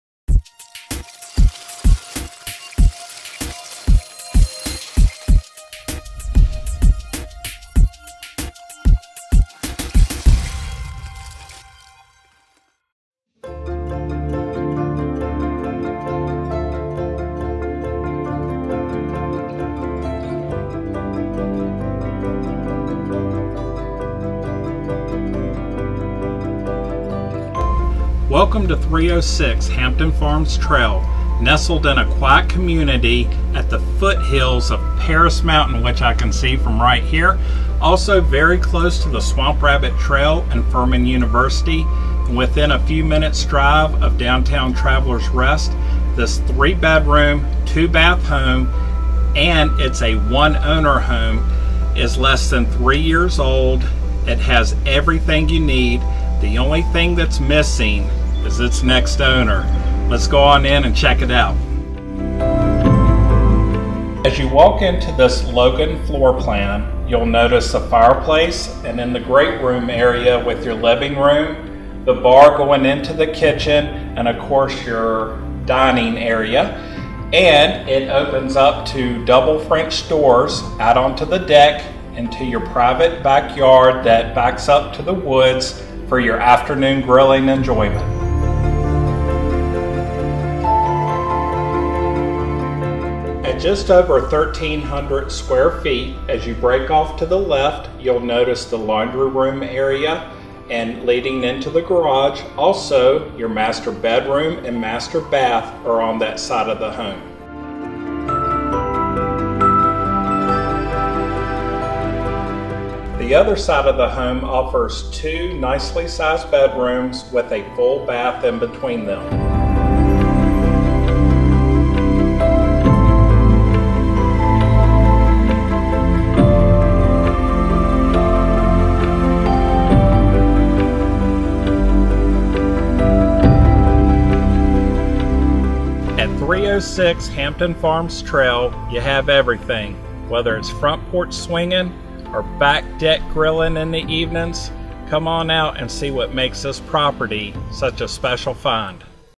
306 Hampton Farms Trail, Greenville, SC, 29617
Great one level, one owner home located a short distance to the Swamp Rabbit Trail and Furman University and within a few minutes drive of Downtown Travelers Rest. This charming, well-maintained home is less than 3 years old and features an open Logan Floor Plan with 3 bedrooms, 2 baths, and a large walk-in crawlspace. The home has also been freshly painted throughout most of the interior. The living room boasts a gas fireplace and opens up to the kitchen for easy entertaining. The kitchen is large and provides ample cabinet space, black appliances, and an eat-in kitchen. The master includes a trey ceiling, double sinks in the master bathroom, walk-in closet, and separate shower/ garden tub. Outside, enjoy your mornings on the front porch, or cool evenings out on the back deck with the privacy of a backyard that backs up to the woods. This won't last long! Schedule your showing today!